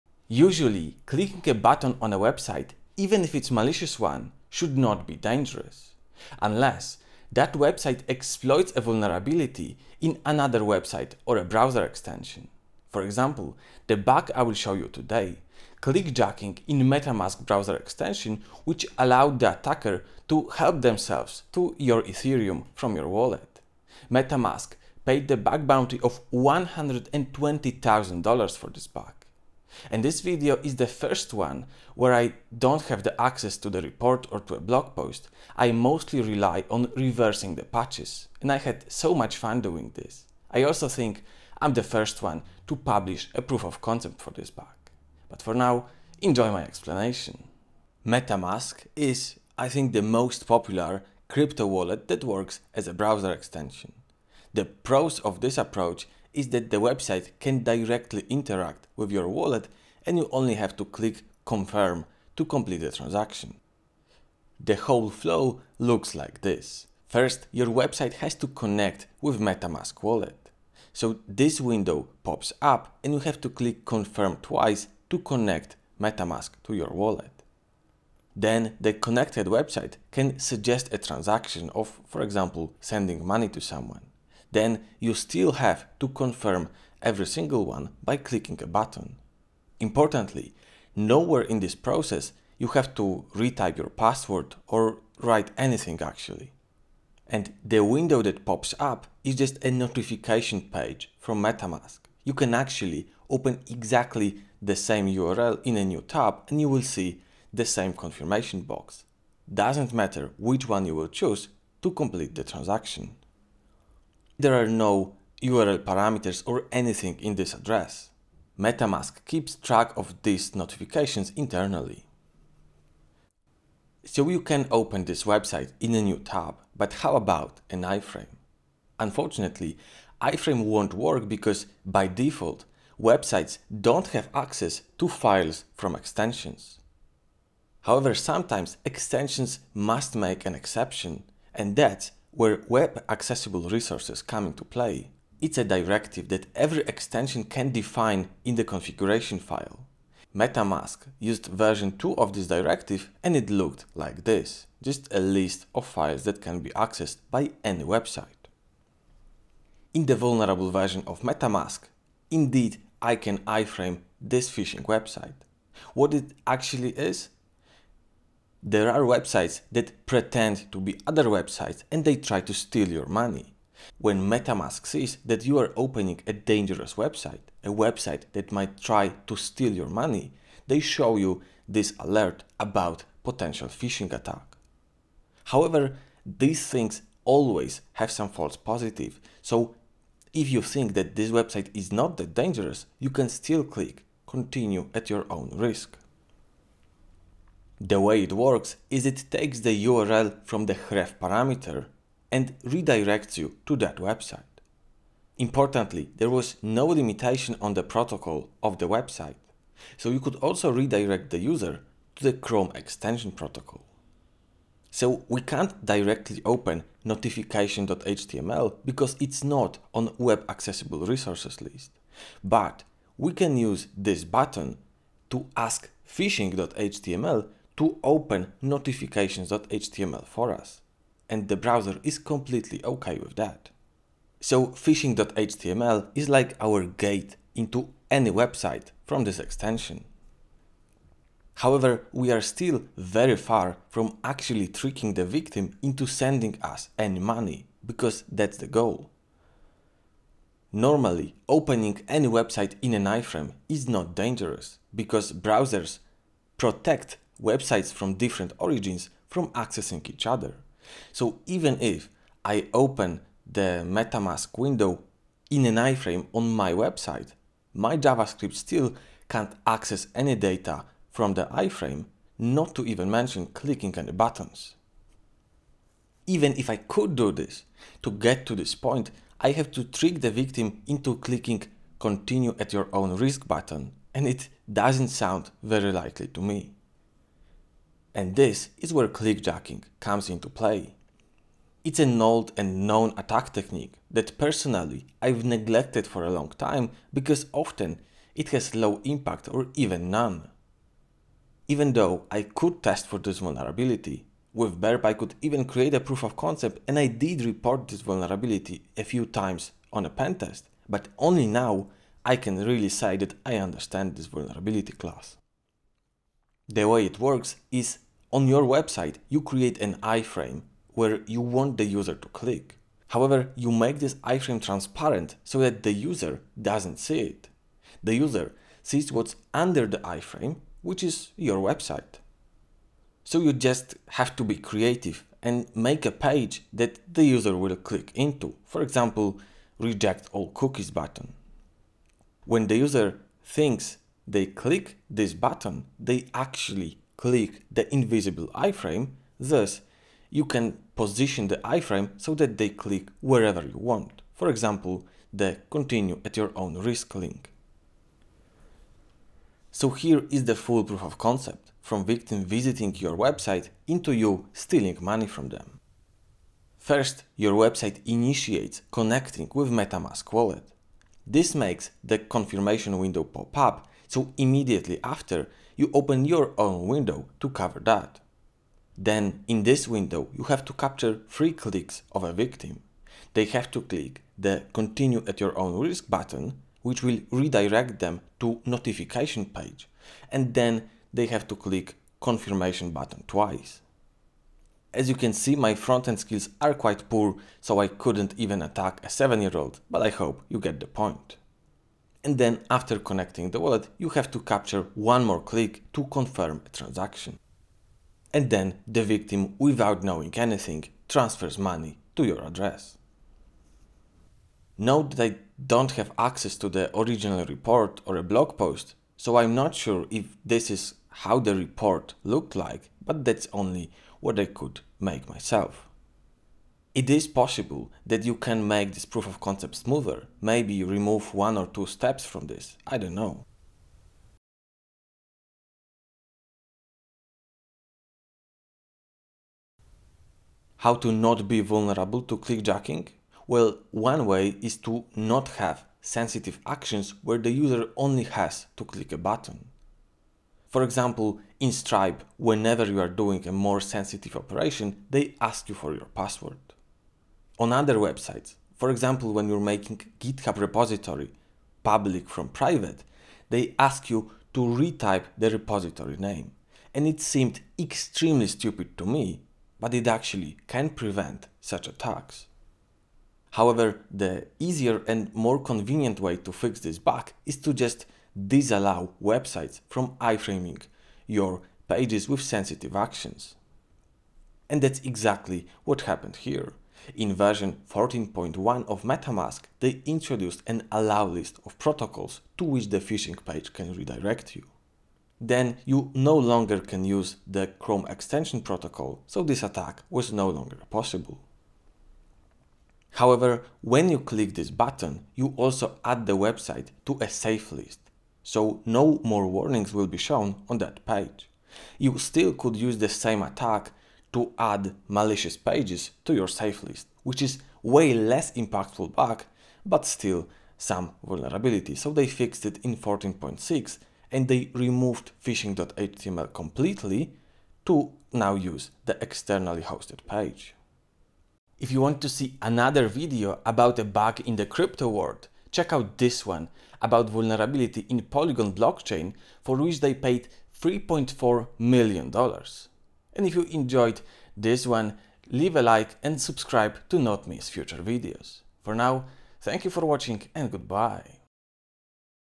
MetaMask - stealing ETH by exploiting clickjacking - $120,000 bug bounty
This video an explanation of a clickjacking bug in MetaMask that allowed the attacker to steal victim's Ethereum with a few clicks. Metamask paid $120,000 bug bounty for it to United Global Whitehat Security Team (UGWST), including René Kroka and José Almeida.

Timestamps:
00:00 Intro
00:47 What is MetaMask and how it works?
02:07 What are Web Accessible Resources?
04:11 Clickjacking - what is the impact of iframing a website?
06:00 Proof of Concept
07:20 How to prevent clickjacking?